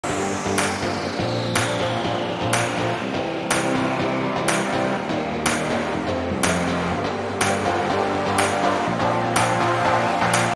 Sttylme - Traditional Maroon Clutch
Price: ₹ 1300 /-

➡️ Synthetic material stylish clutch
➡️ Clasp Closure
➡️ Dimension: (LXH) 8.5 X 5 Inches
➡️ One Main compartment.